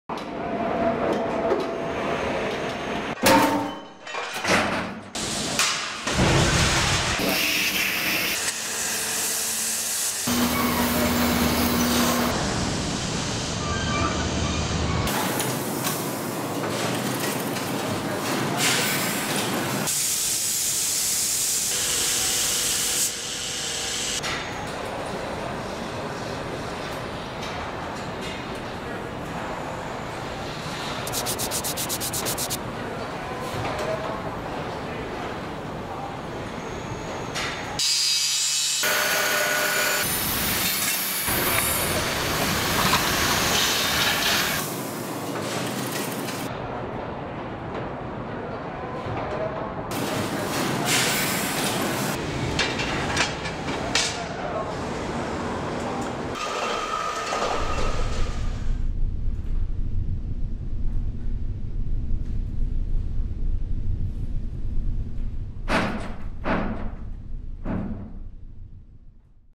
JOTUL glissez vous dans les coulisses de notre usine norvégienne
Jotul : découvrez l'usine en Norvège et la fabrication des produits Jotul ... puis venez les voir en vrai dans nos magasins : Atre et Loisirs 73 à Arbin, Atre et Loisirs 38 à St martin d'Hères et Jotul Chambéry 73.